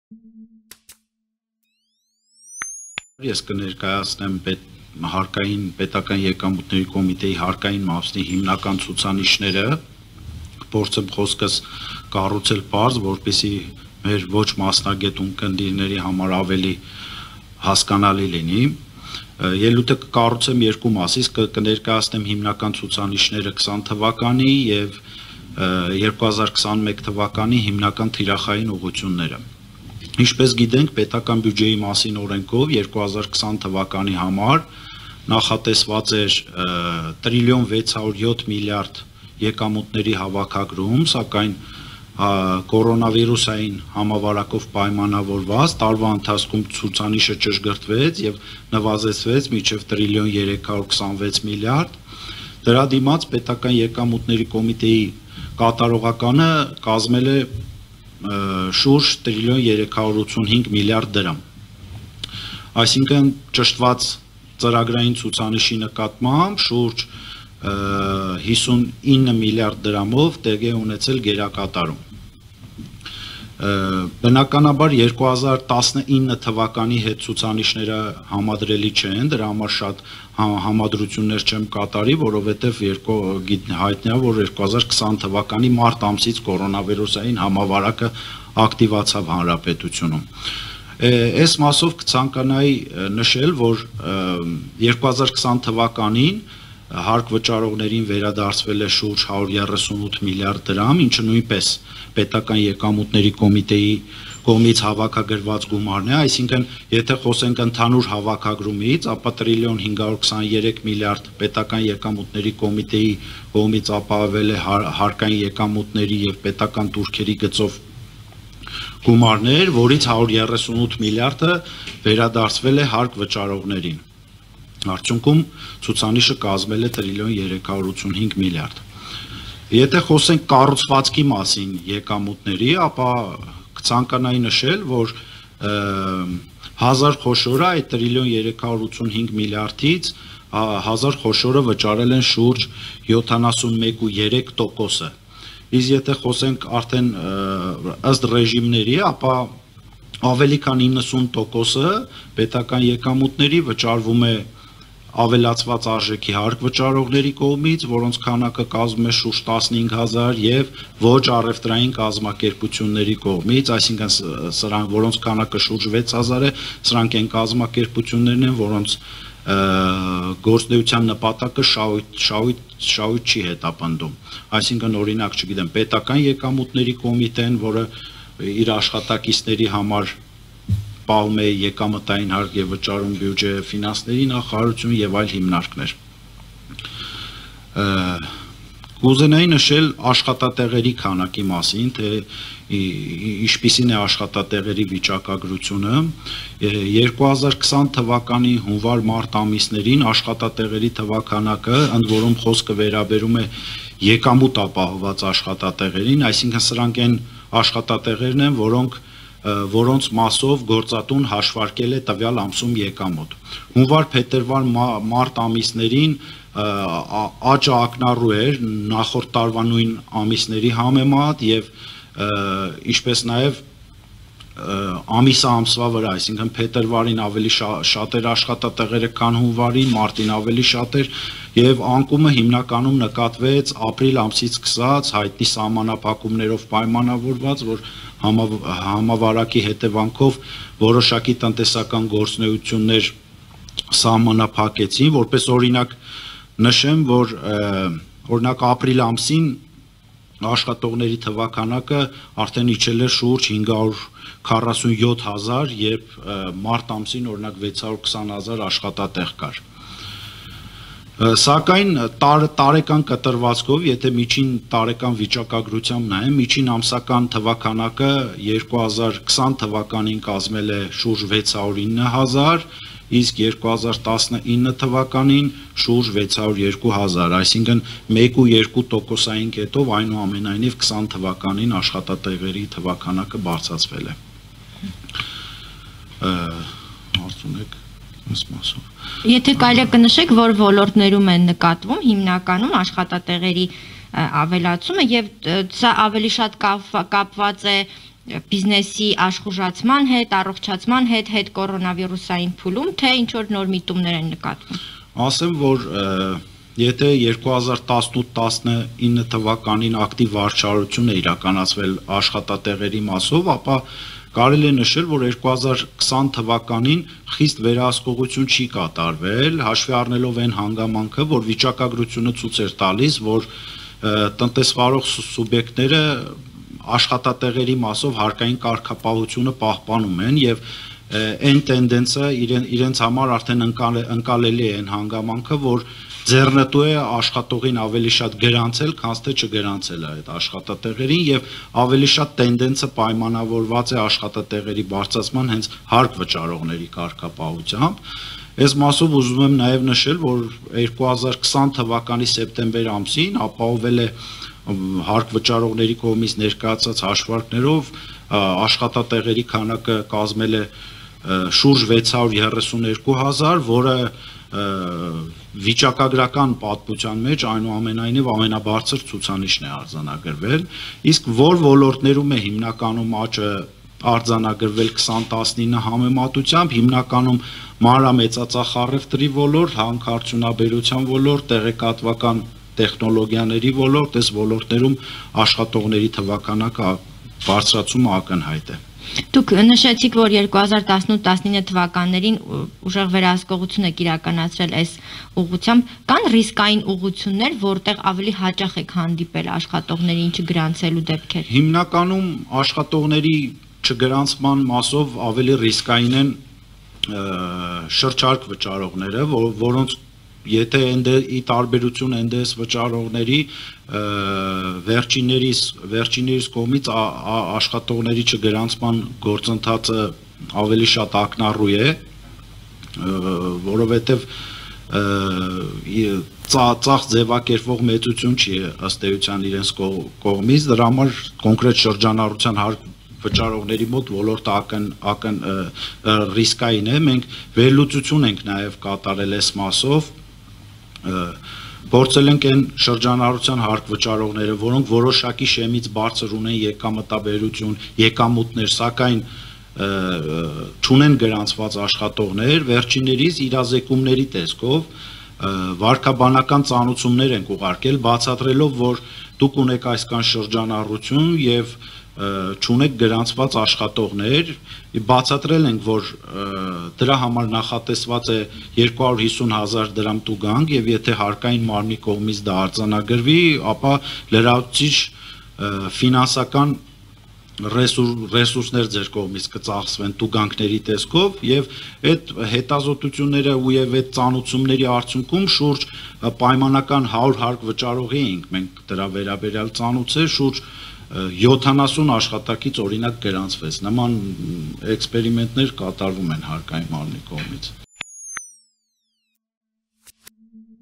Աշխատատեղերի անկումը սկսել է նկատվել 2020 թվականի ապրիլ ամսից. Կարեն Թամազյան
2020 թվականին հարկ վճարողներին վերադարձվել է շուրջ 138 միլիարդ դրամ. ՊԵԿ ներկայացուցիչ